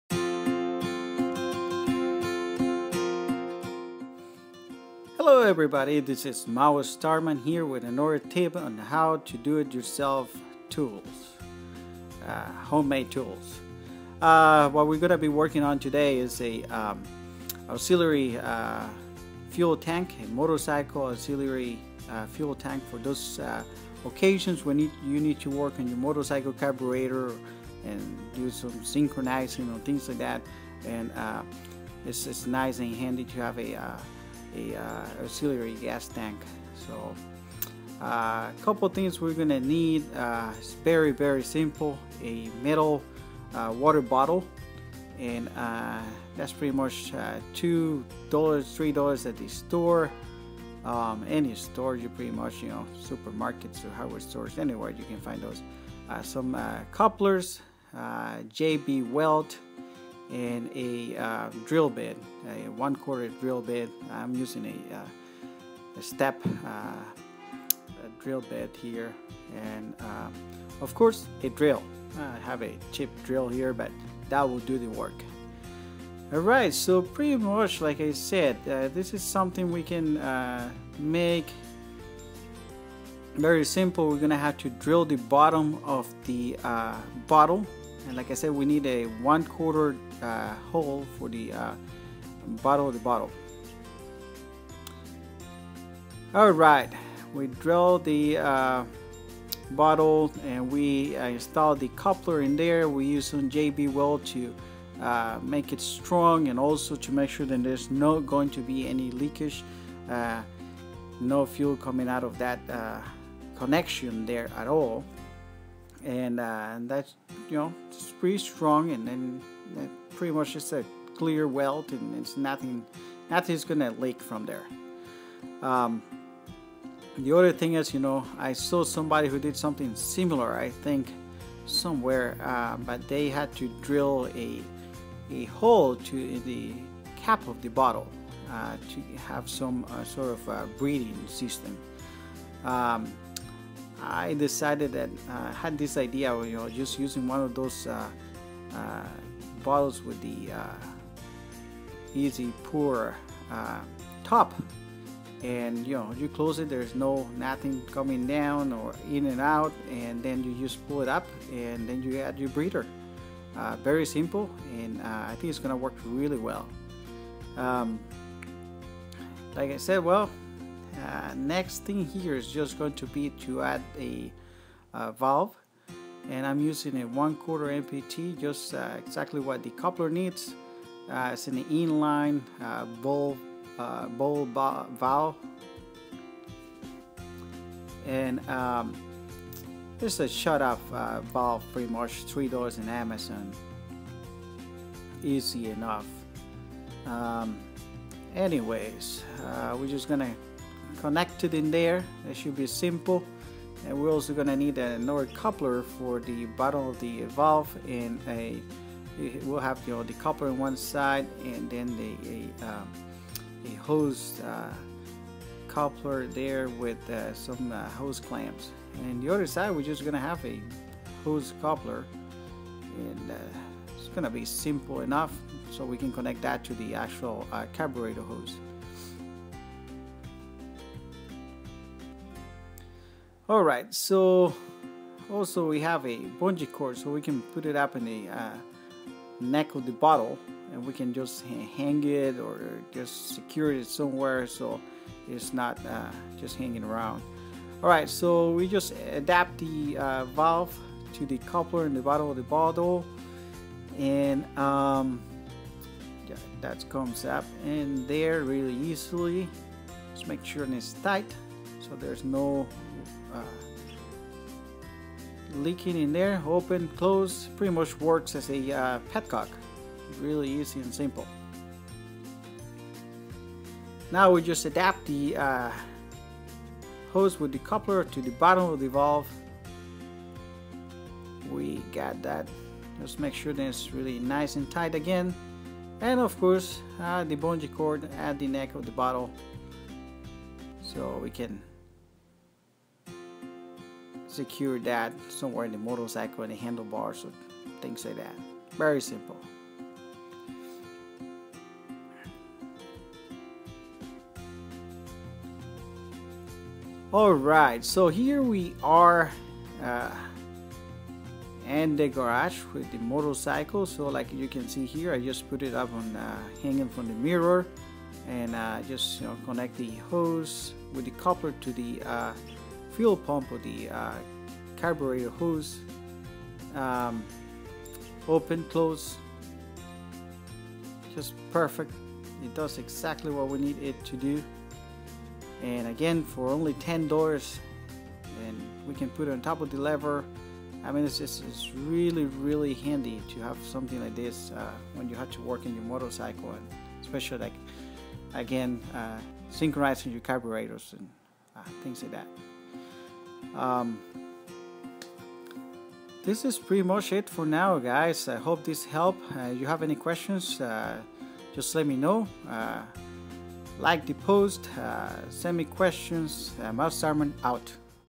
DIY- Motorcycle Auxiliary Fuel Tank
This is A DIY video on how to make an auxiliary fuel tank to work on your motorcycle.
Very handy to have around and it can be made for less than $10 compared with the stuff out there for over $50
Thank you -- Maustarman